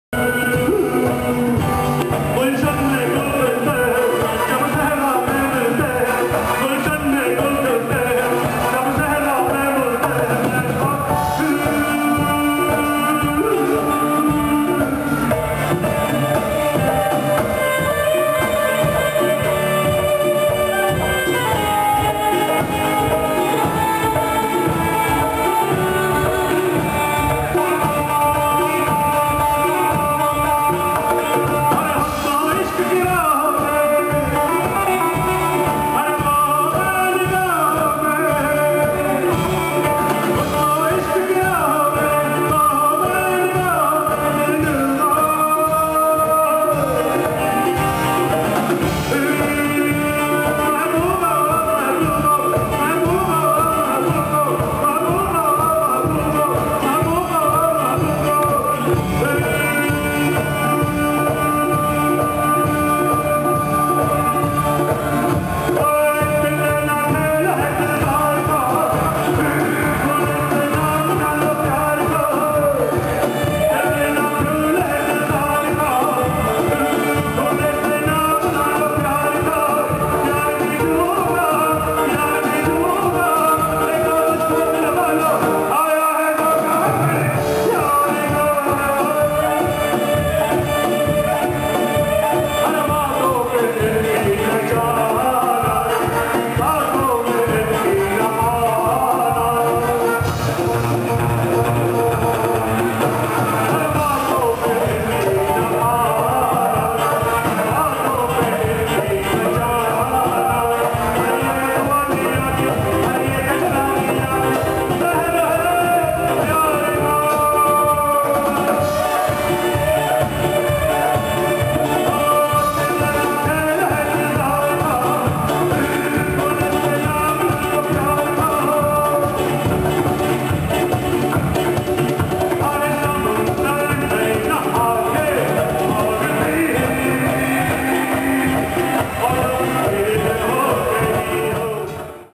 Jolly Mukherjee at Platinum Jubilee, BES School
Jolly Mukherjee, alumnus 1972 of BES School, Mumbai  sings at Platinum Jubilee program at Shanmukhananda Hall, on 16th October, 2008